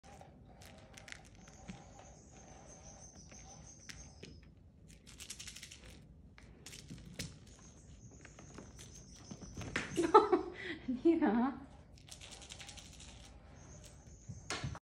Beautiful Siamese Cat Playing Hard!
Hi furriends,

Here is pretty Nina with her cute head shake watching the mouse... and the ultimate fall from playing too hard. I love this purrfect girl.